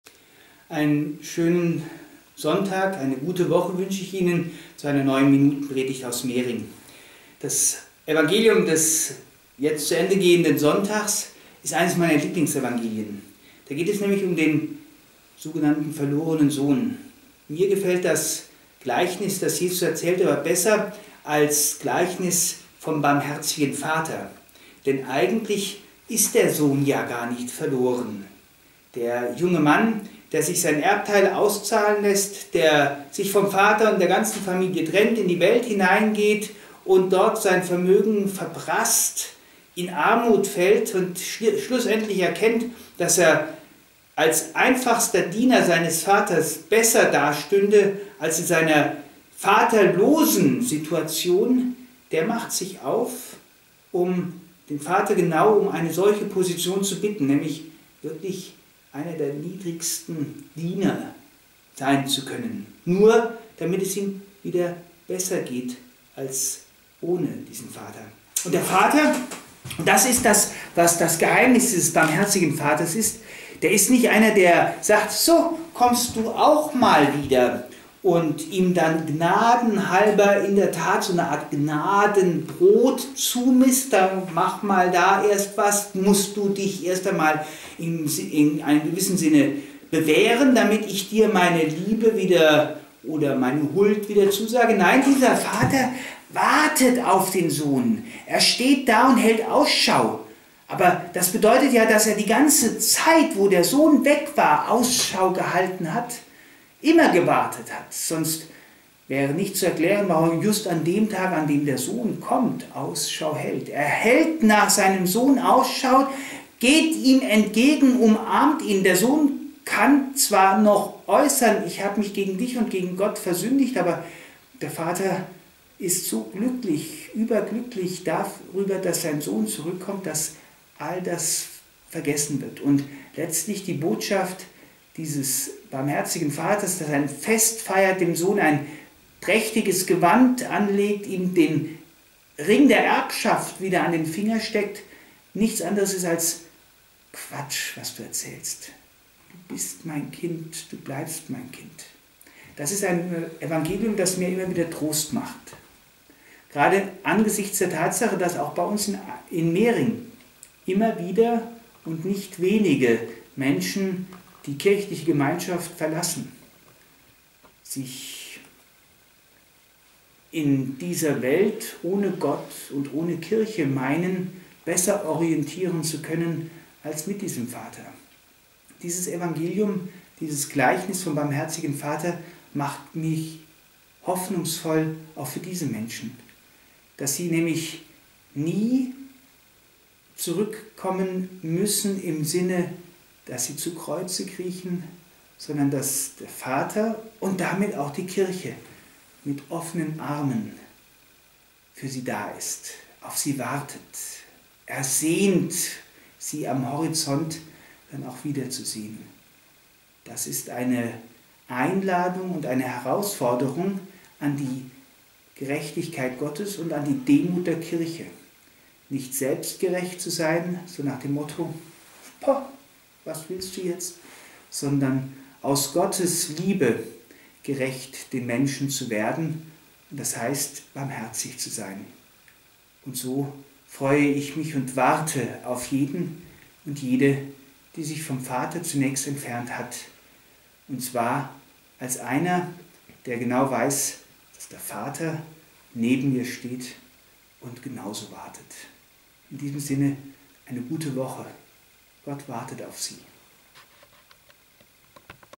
Minutenpredigt zum 4. Fastensonntag 2013
Minutenpredigt zum Sonntag,10. März 2013 von Herrn Pfarrer Thomas Schwartz von St. Michael Mering, siehe auch: